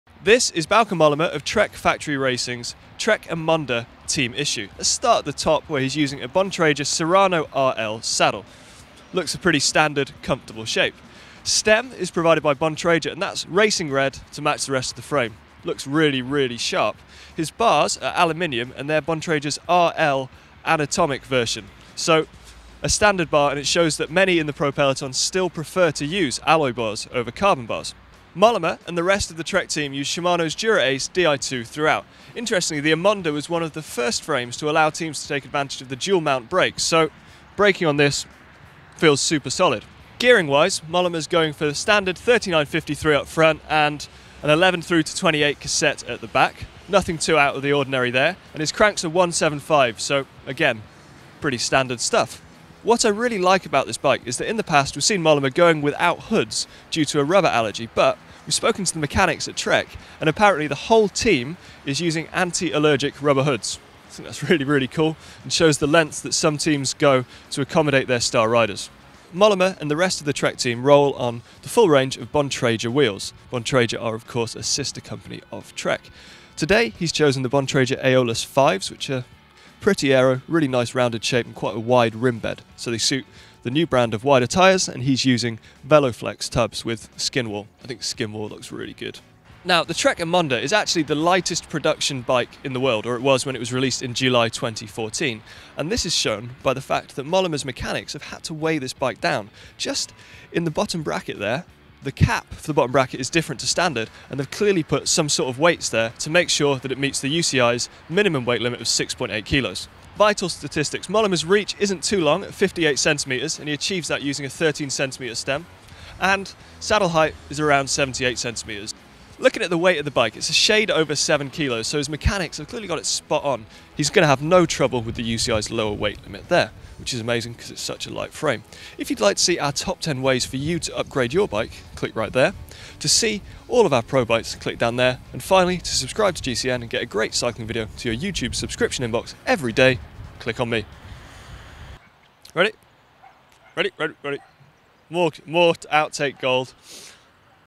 Bauke Mollema's Trek Émonda
The Trek Factory Racing riders have the choice between three Trek frames. Team leader Bauke Mollema goes for an Émonda.

Bauke Mollema has matured into a solid GC contender for the Grand Tours. We took a look at his bike at the Tour of the Basque Country, where he showed great form before later crashing out.

Mollema's bike is fully specced out with Bontrager finishing kit. On this day, he was using Bontrager's Aeolus 5 wheels. Shifting and braking is provided by Shimano, with the ever reliable Dura Ace Di2 throughout.

This bike is so light that Mollema's mechanics actually had to weigh it down - check out the weights in the bottom bracket.